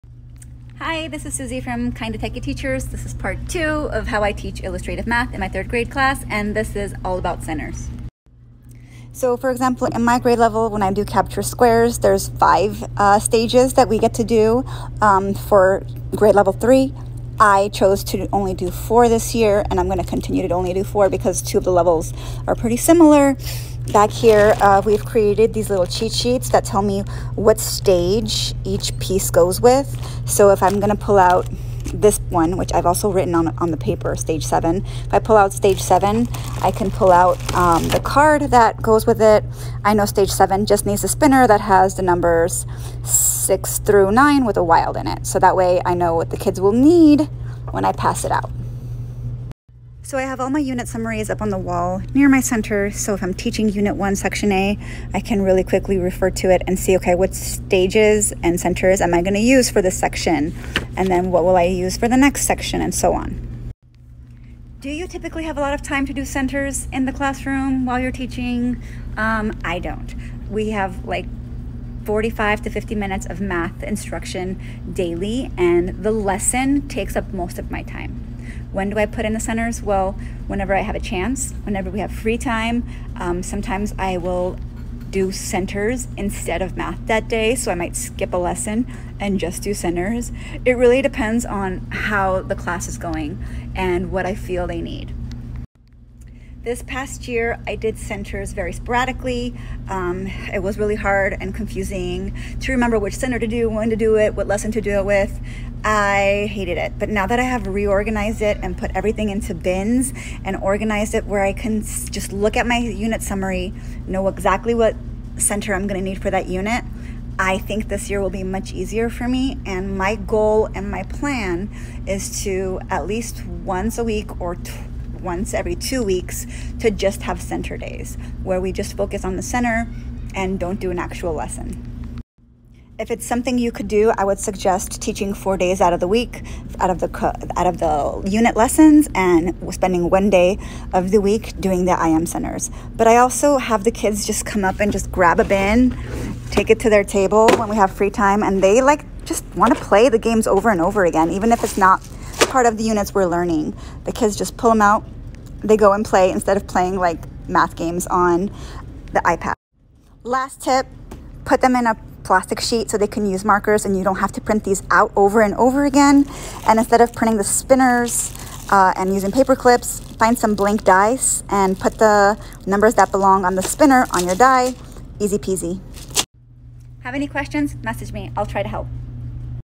How to Organize Math Centers Inspired by Illustrative Mathematics®
In this video, I walk through how I organize math centers inspired by IM® in my classroom. This process helps me stay organized, but also makes it easy for me to access centers and remember to use them often! Even my students can access these and play with them when they are finished with other work.